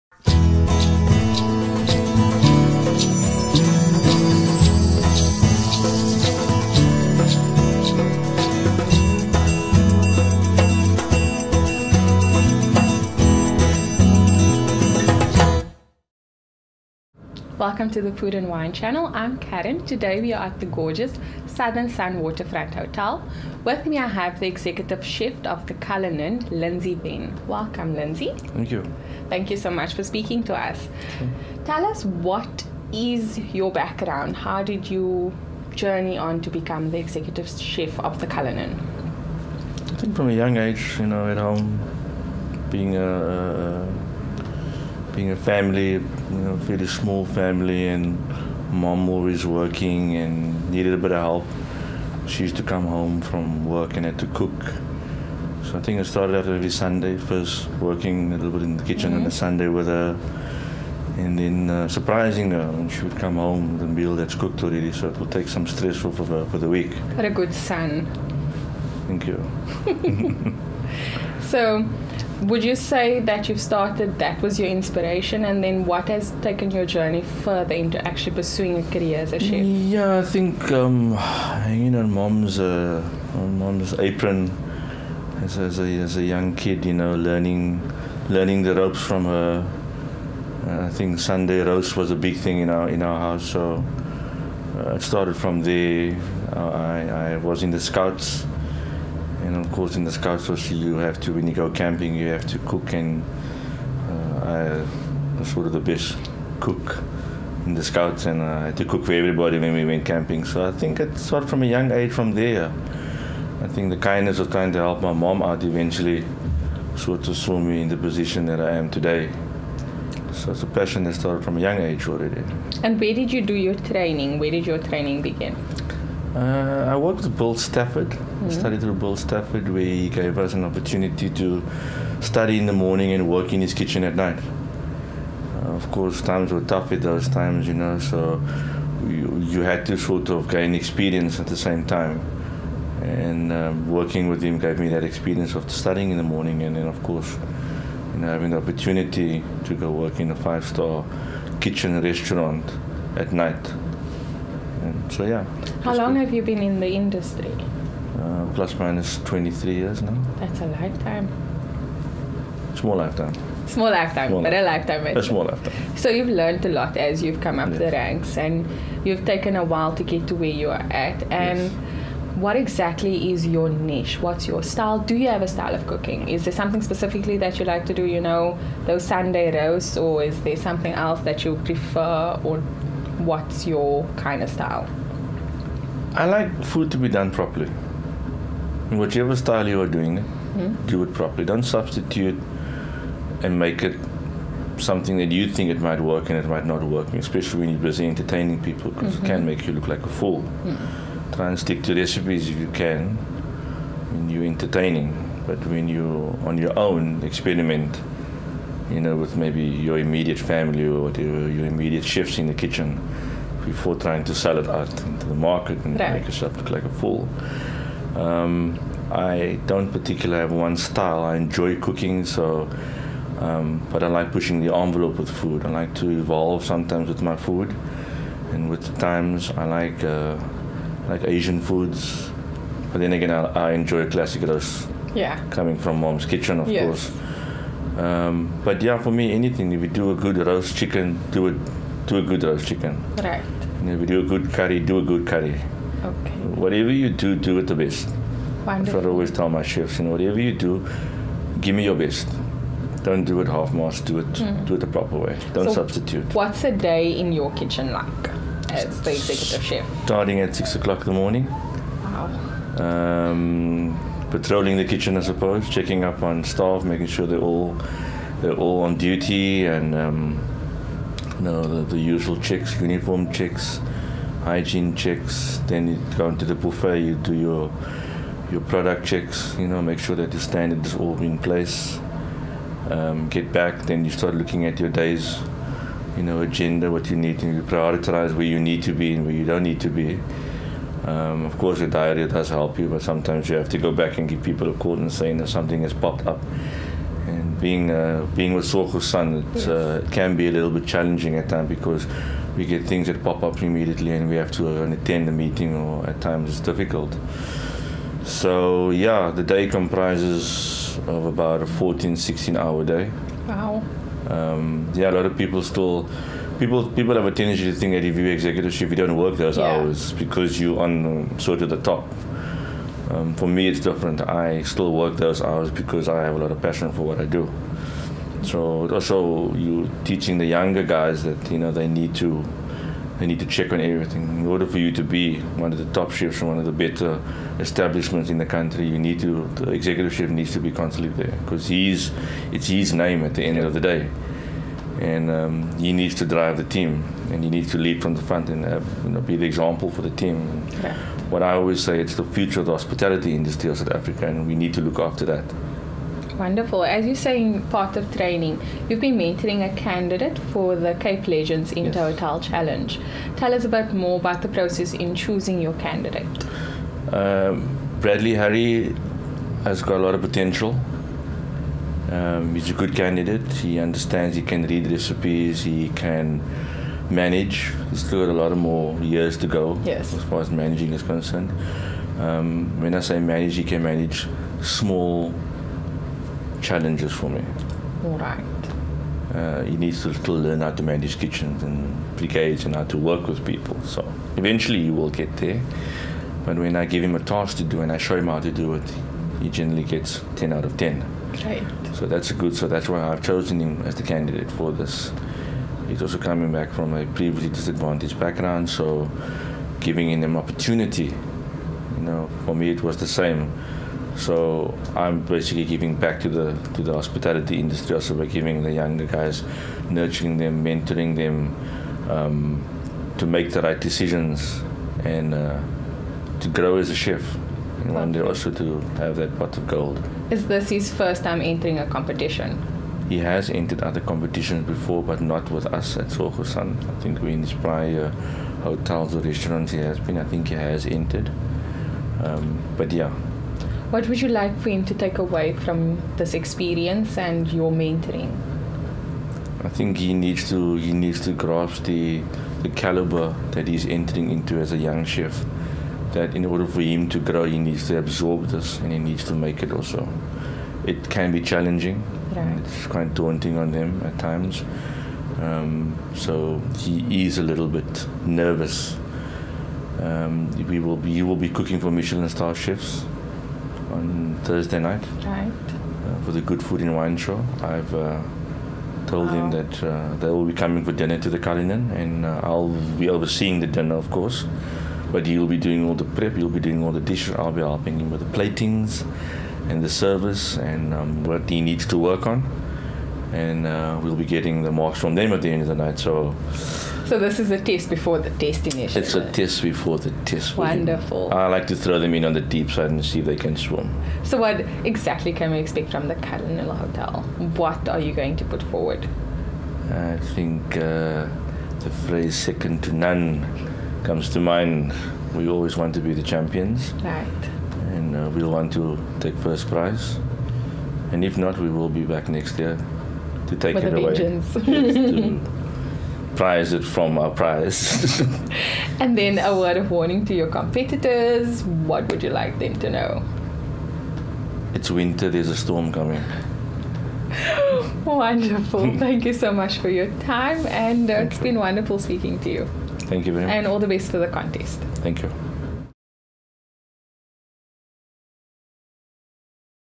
Interview with Lindsay Venn, Head Chef of Southern Sun Hotel Cullinan V&A WTF
Carrie Goliath profiling the head exec chef of the Southern Sun Hotel Cullinan V&A WTF in Cape Town on his role and contribution on the Cape Legends inter hotel Challenge 2014. An initiative that is a powerful entity for the upliftment, training and nurturing of the thousands of young people that come into the hospitality industry creating an opportunity for the participating hotels to become a unified force. In 2014 Cape Legends Inter Hotel Challenge has gone nationwide, launching in Cape Town, Gauteng and KwaZulu-Natal with 21 hotels coming on board and seeing a huge growth in this exciting project.